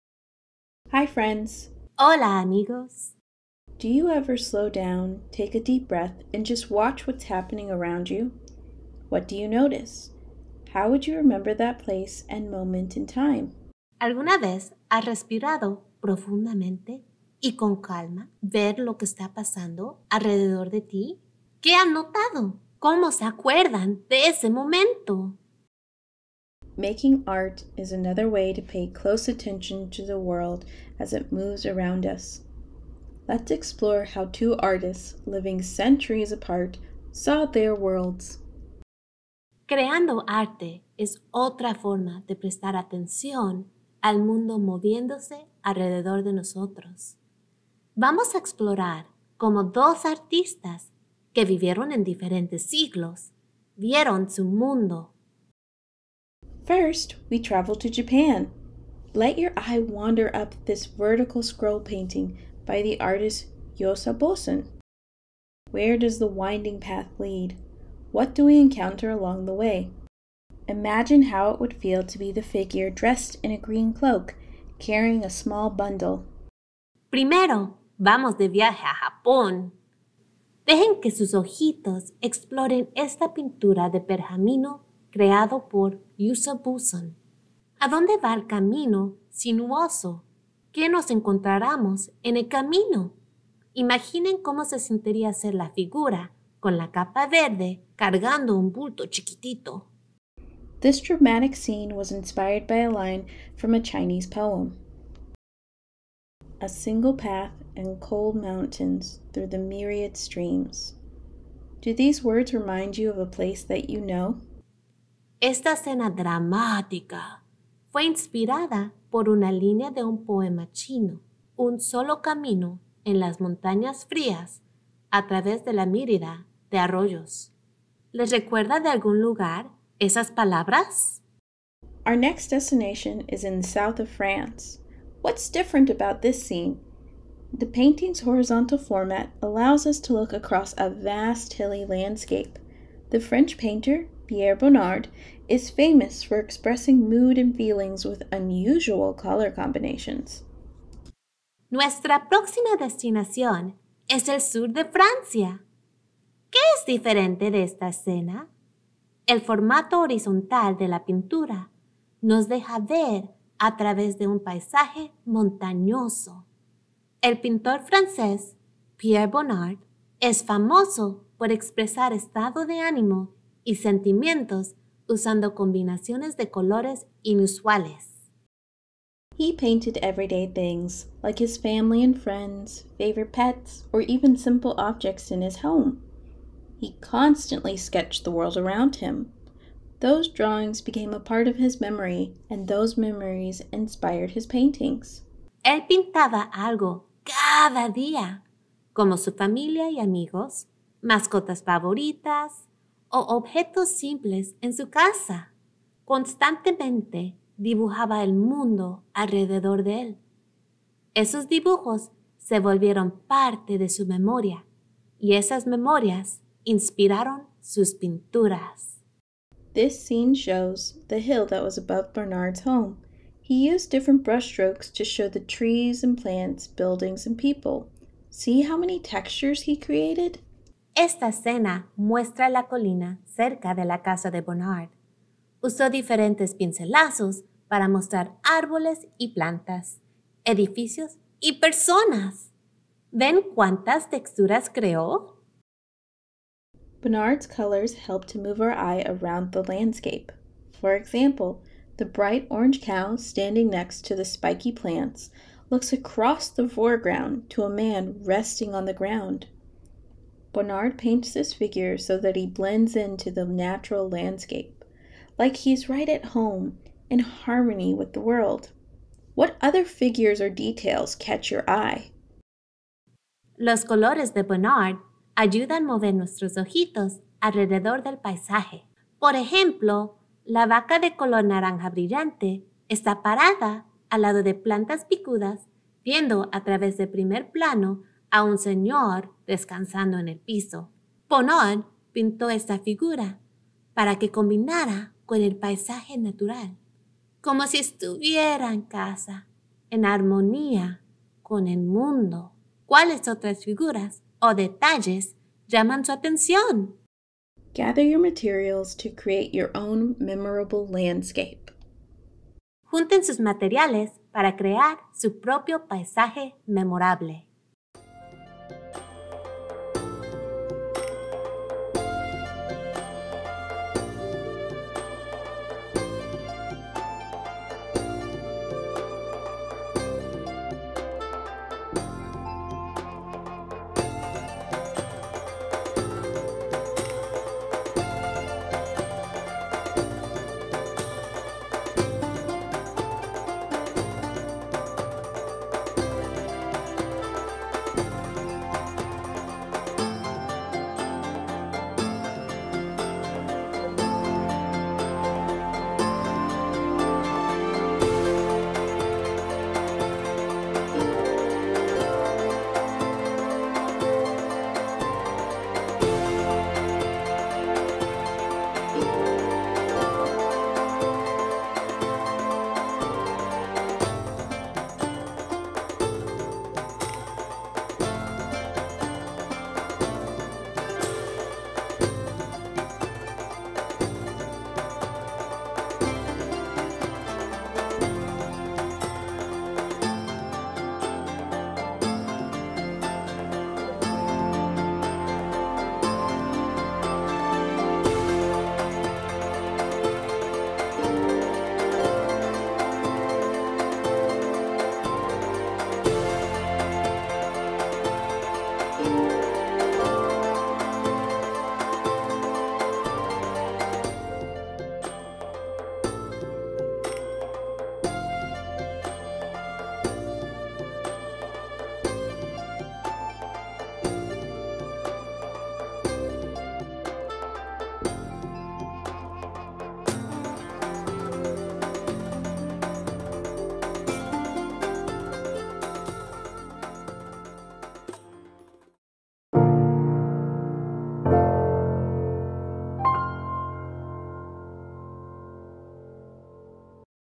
How I See the World: Landscapes, with guest artist Lillian Young
Making art helps us to pay close attention to the world around us! Landscapes from Japan and France will inspire watercolor sketches of our favorite places.

Como veo el mundo: Paisajes, con artista invitada Lillian Young

¡Creando arte nos ayuda prestar atención al mundo alrededor de nosotros! Paisajes de Japón y Francia inspiraran dibujos en acuarelas de nuestros lugares favoritos.

Learn more about The Kimbell Art Museum's "Spring Break Art Extravaganza"